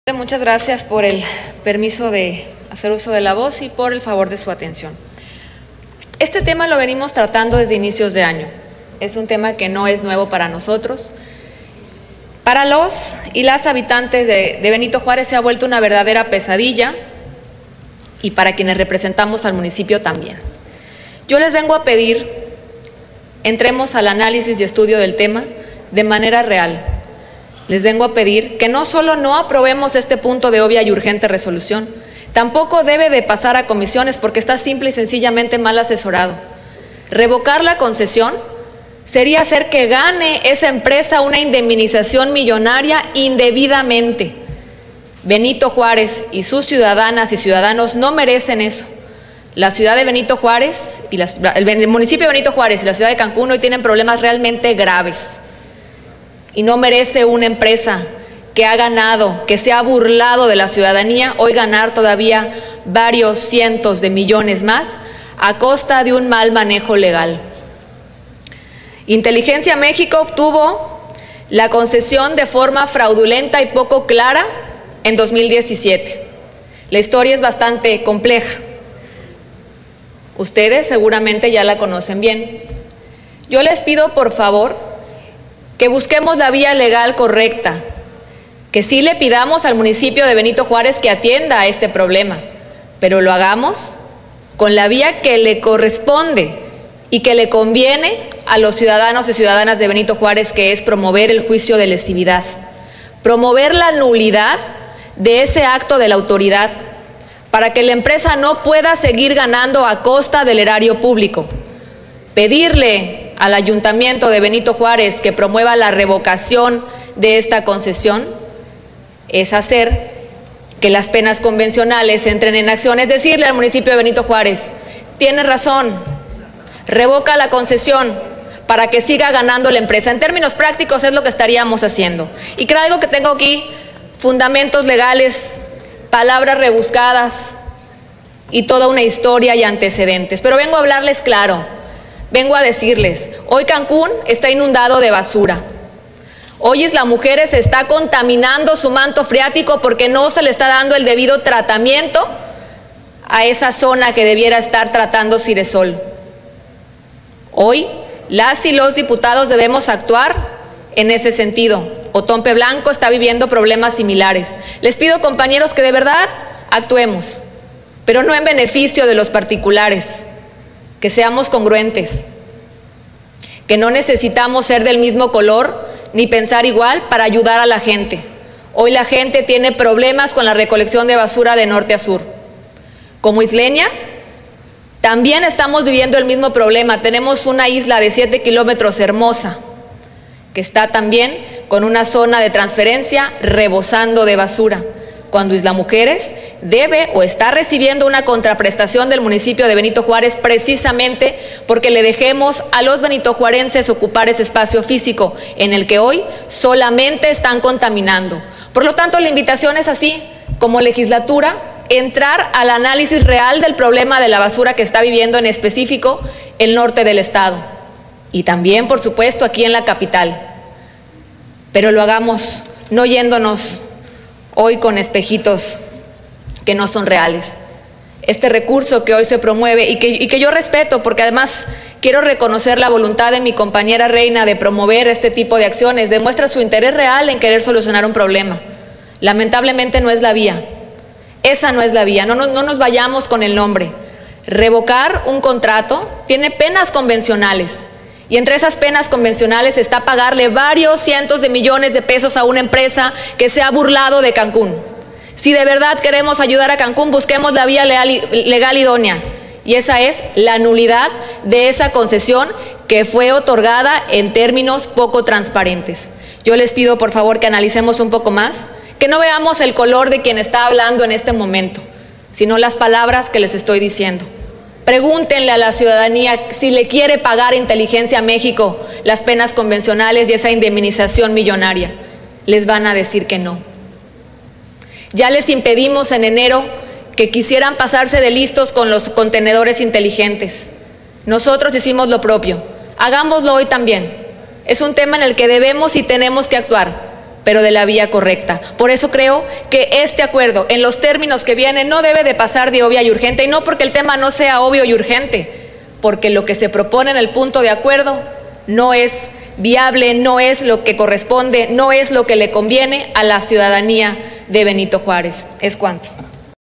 Intervención de la Dip. Teresa Atenea Gómez Ricalde. PAN (29/Oct/2020)
Intervención de la Diputada Teresa Atenea Gomez Ricalde, del PAN, en relación a la Proposición con Punto de Acuerdo de obvia y urgente resolución, mediante el cual la Honorable XVI Legislatura del Estado Libre y Soberano de Quintana Roo, solicita a: 1.- Cabildo del Ayuntamiento de Benito Juárez, Quintana Roo, para que en el ámbito de sus competencias realicen: 1.- Los actos jurídicos idóneos a efecto de REVOCAR la concesión otorgada a la empresa denominada INTELLIGENCIA MÉXICO S.A. DE C.V. para la recolección y destino final de los residuos sólidos en el Municipio de Benito Juárez, Quintana Roo (CANCÚN); sin perjuicio de la hacienda pública municipal y: 2.- Realicen de forma inmediata todas las acciones necesarias para que se desarrolle de manera eficaz el servicio de referencia.